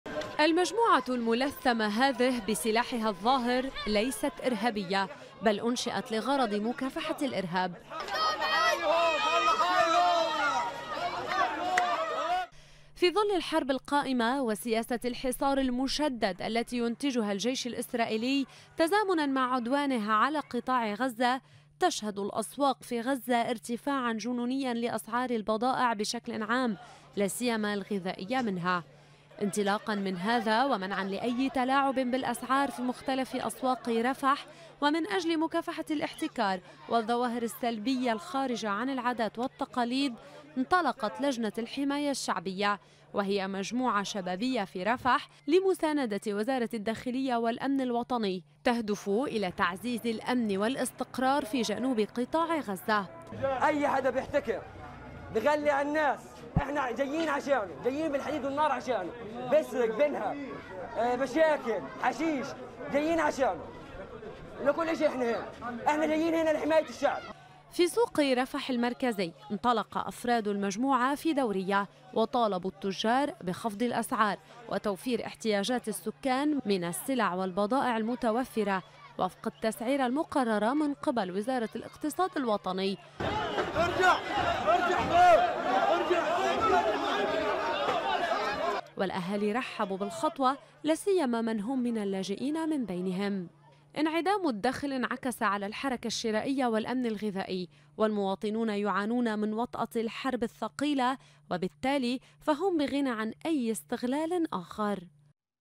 لجنة الحماية الشعبية | المجموعة الملثمة بسلاحها ليست ارهابية بل تكافح الارهاب
ملثمو رفح يجولون الأسواق في غزة ويطالبون التجار بخفضِ الأسعار وتوفيرِ احتياجاتِ السكان. تفاصيل اكثر عن لجنة الحماية الشعبية،
تقرير ميريلا بو خليل